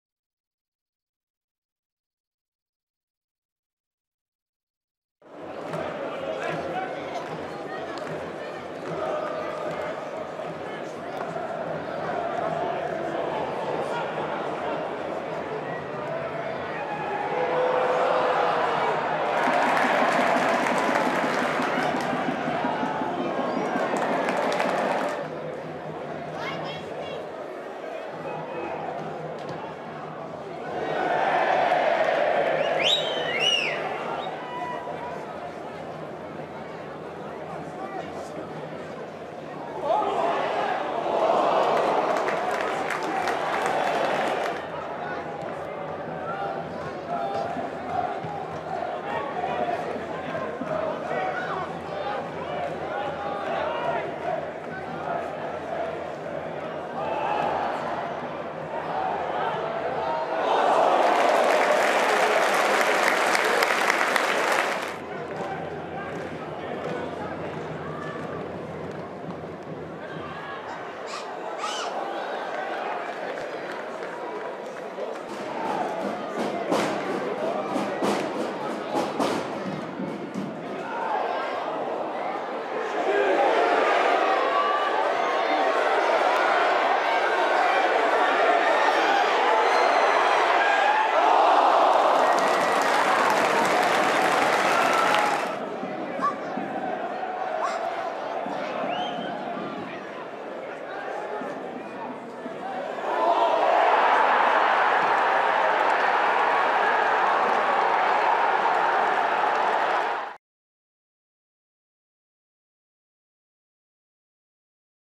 HIGHLIGHTS: Carlisle United 0 Exeter City 1 (9/4/22) EFL Sky Bet League Two
Padraig Amond's late header was enough to earn the Grecians a big three points at Brunton Park.